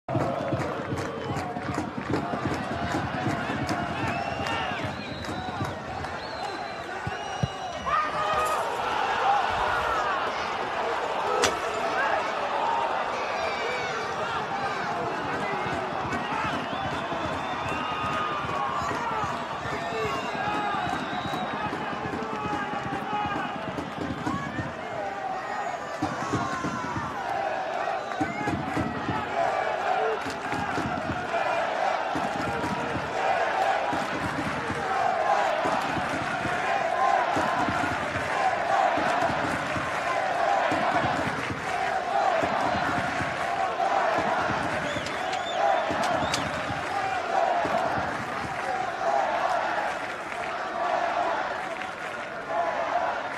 Golo Sorriso: Estrela Amadora 0-(1) Famalicão (Liga 24/25 #2)
Gustavo Sá trabalha bem na esquerda, cruza atrasado em cima da linha de fundo, e Sorriso, ao primeiro poste, só teve de encostar!